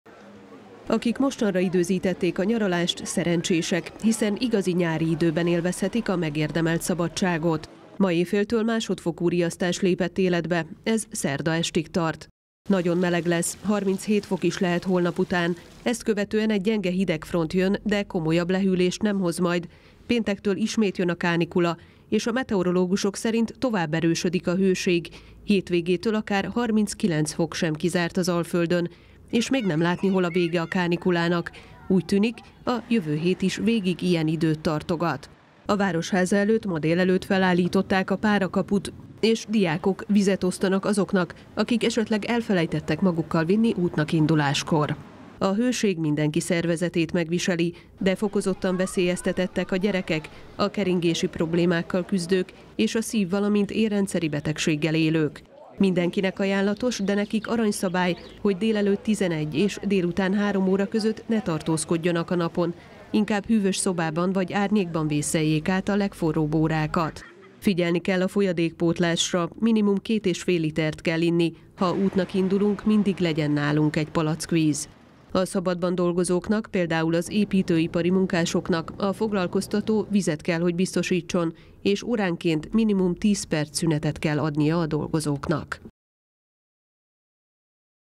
Itt a hőség és marad is még egy jó darabig
Az idei nyár eddigi legforróbb és leghosszabban tartó kánikulai időszaka kezdődött el. Az egész országra másodfokú hőségriasztást adott ki a tisztifőorvos. Szerdán 37 fok is lehet, aztán érkezik néhány fokos lehűlés, és péntektől újra visszatér a 35 fok, sőt akár még annál is melegebb is lehet a hétvégétől. A Városháza előtt párakaput állítottak fel és vizet osztanak a járókelőknek.